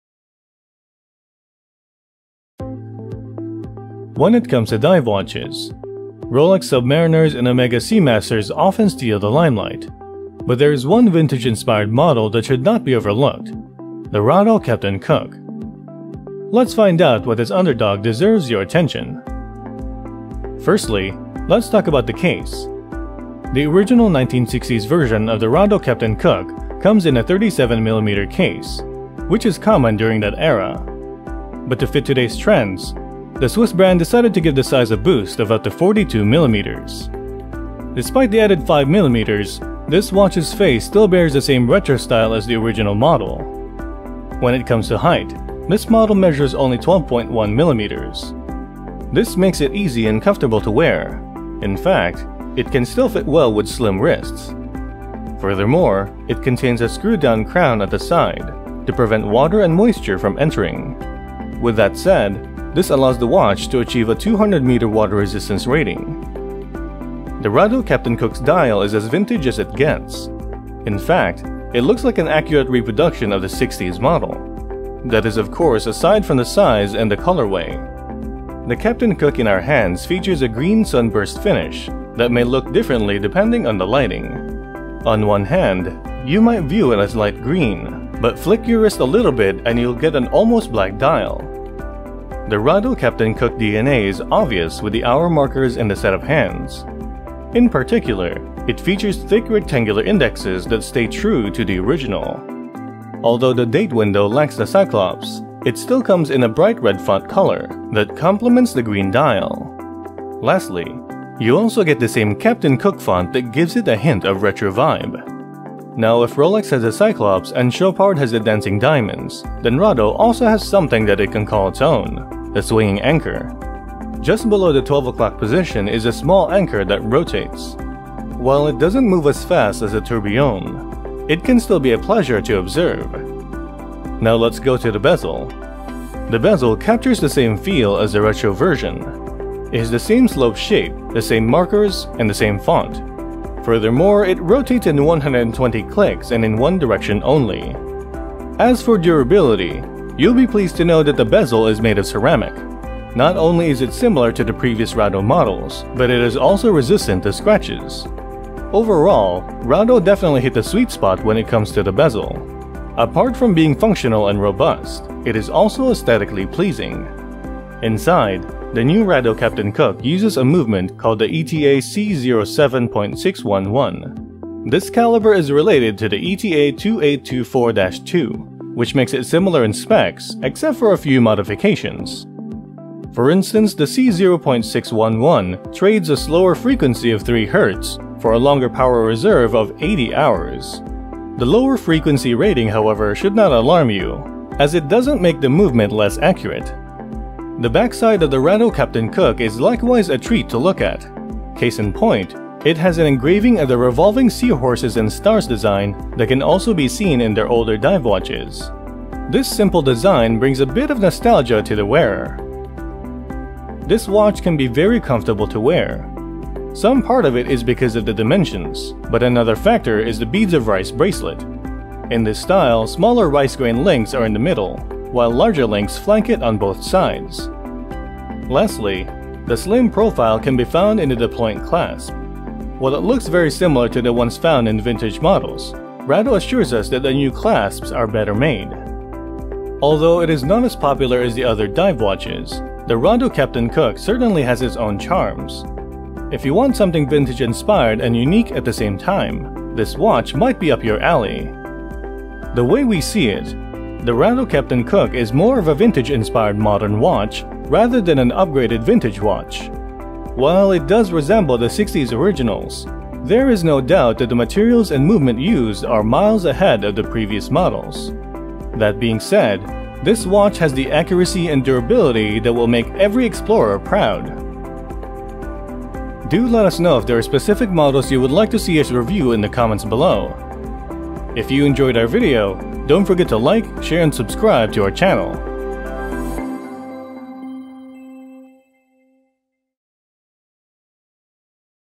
TWC Reviews: Rado Captain Cook Dive Watch
Rado Captain Cook Dive Watch

When it comes to dive watches, Rolex Submariners and Omega Seamasters often steal the limelight. However, there is one vintage-inspired watch model that should not be overlooked — the Rado Captain Cook.

Nowadays, calling a watch a diver does not necessarily mean that people use them for their intended purpose. However, the Rado Captain Cook was definitely made for diving. It has everything a diver would need — 220m water resistance rating, automatic movement, and a unidirectional bezel.

Although it is not as popular as the other dive watches, the Rado Captain Cook certainly has its own charm. If you want something vintage-inspired and unique at the same time, this watch might be up your alley.

While it does resemble the ’60s originals, there is no doubt that the materials and movements used are ages ahead of the previous models. That being said, this watch has the accuracy and durability that will make every explorer proud.

Rado Captain Cook Automatic Green Dial Men's Watch
Ref: R32505313